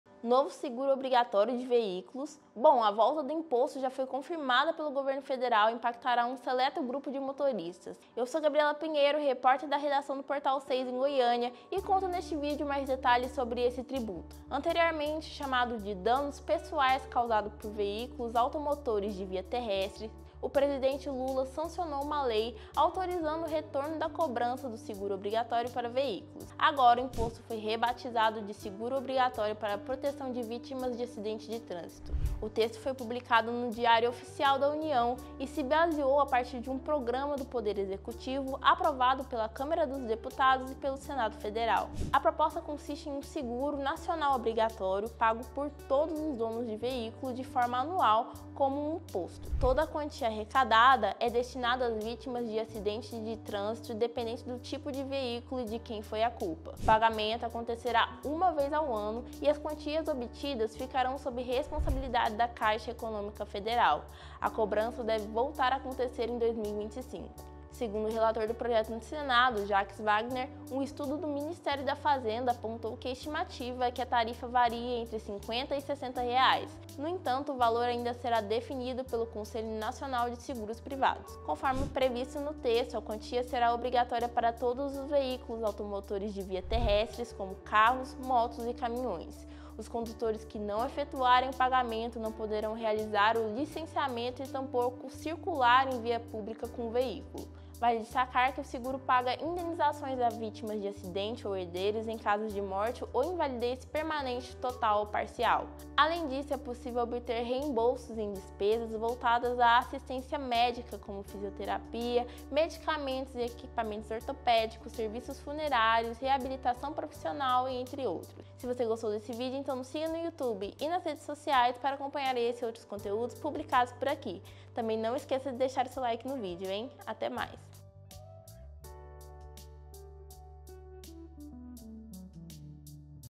Saiba quem deve pagar novo seguro obrigatório de veículos
Bem-vindo ao canal do Portal 6!

Um novo imposto obrigatório para arrecadação de seguros de veículos acaba de entrar em vigor no Brasil e deverá ser pago pelos motoristas.

A ação foi publicado no Diário Oficial da União e se baseou a partir de um projeto do Poder Executivo. Saiba mais!